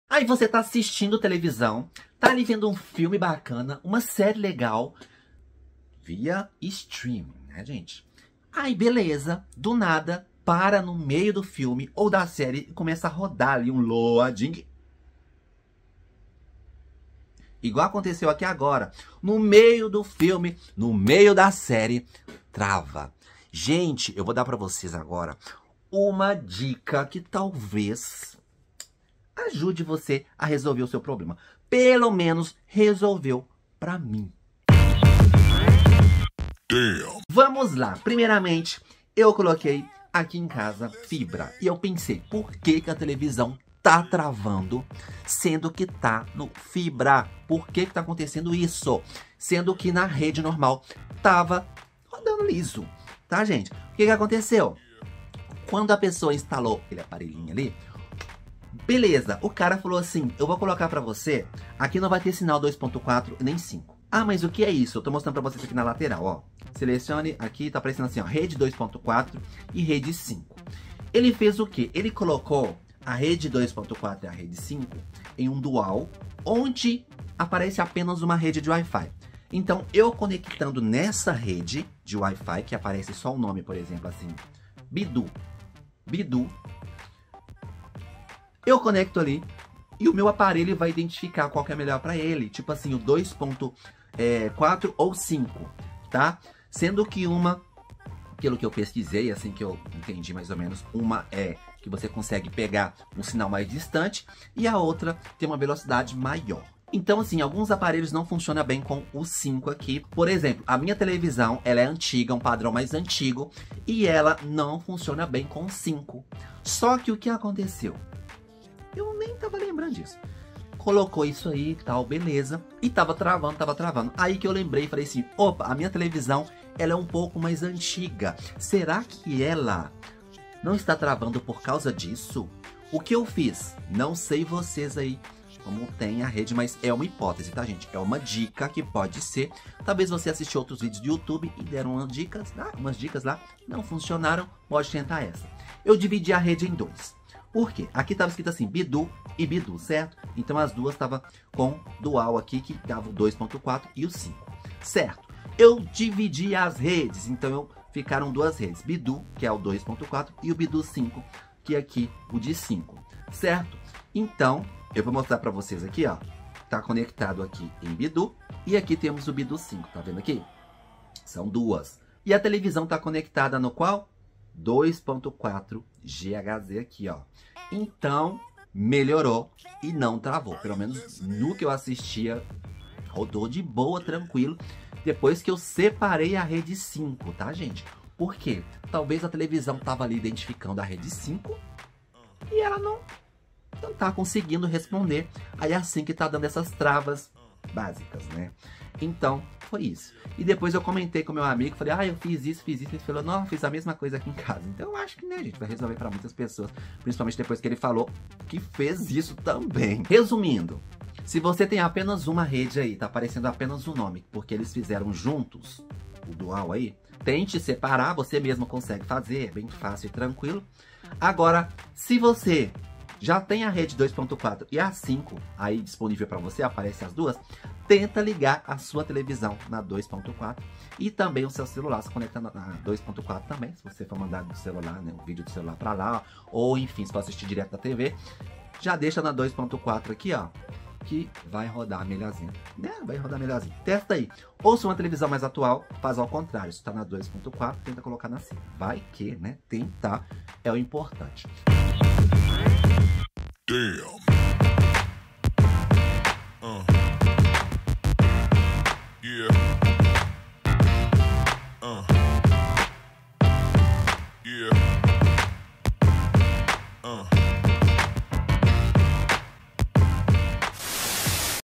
DICA: FILME OU SÉRIE TRAVANDO MESMO COM A INTERNET BOA NA TV ( LOADING, CARREGANDO NO MEIO )
🟢 No vídeo de hoje, você encontra as seguintes informações:

No meio do filme ou série simplesmente começa um loading na televisão mesmo a velocidade da internet estando ótima. Dica que pode ou não resolver.